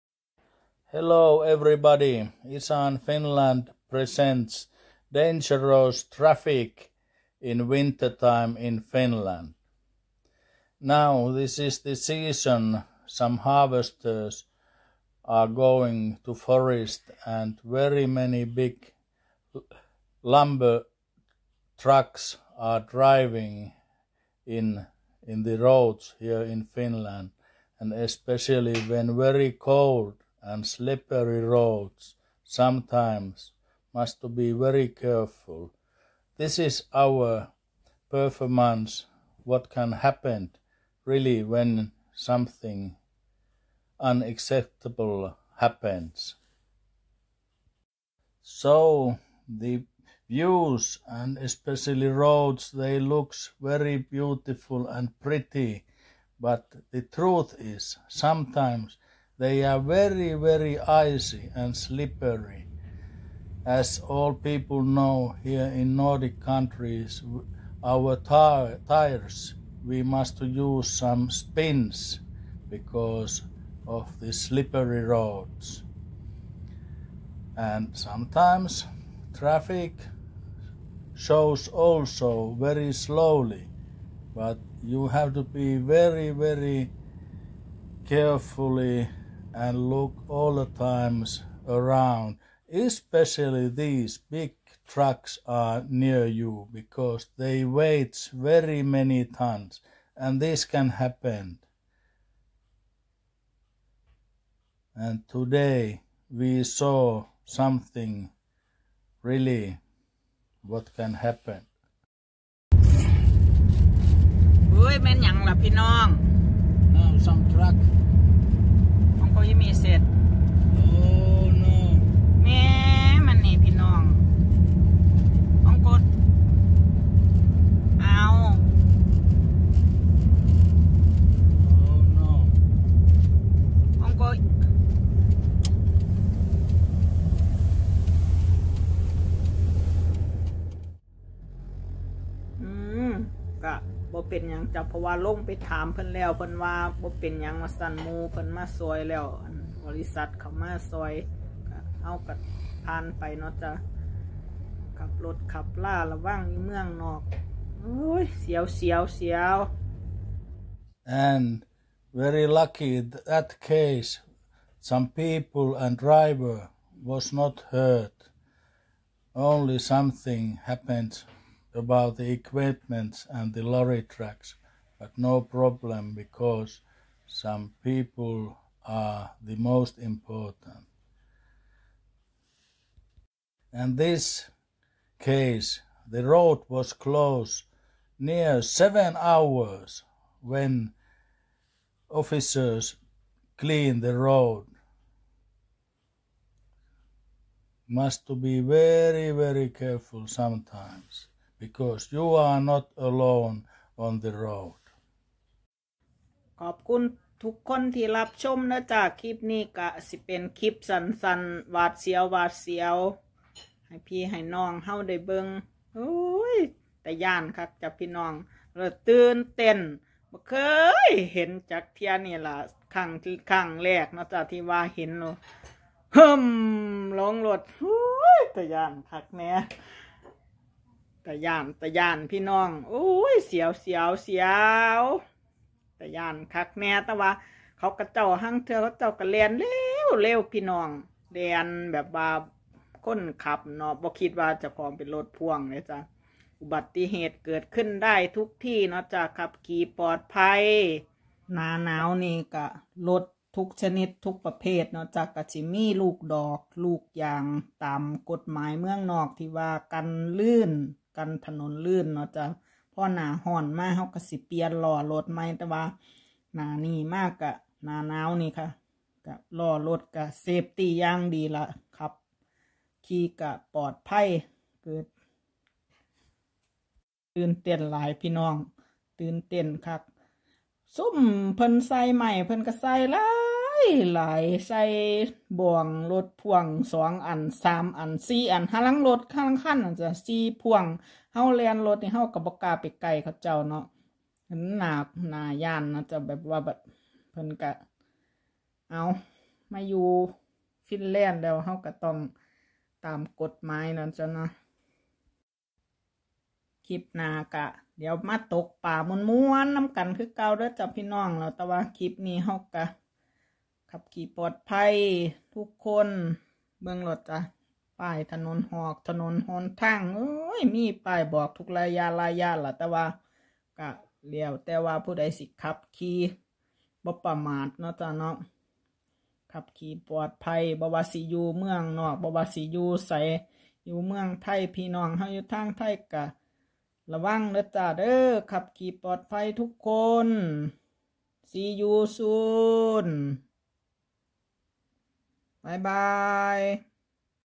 Accident in wintertime#อุบัติเหตุนาทีระทึกใจ#ถนนลื่นหรือขับรถประมาท#รถคล่ำ
meta name="tradetracker-site-verification" content="f0965da77bf19d765068f540e2ab587076c30dc2" / Real-life and Lifestyle in The Authentic Small Organic Farm in Finland. Winter accident. Sometimes happened something not should, roads are narrow, weather conditions are bad, etc.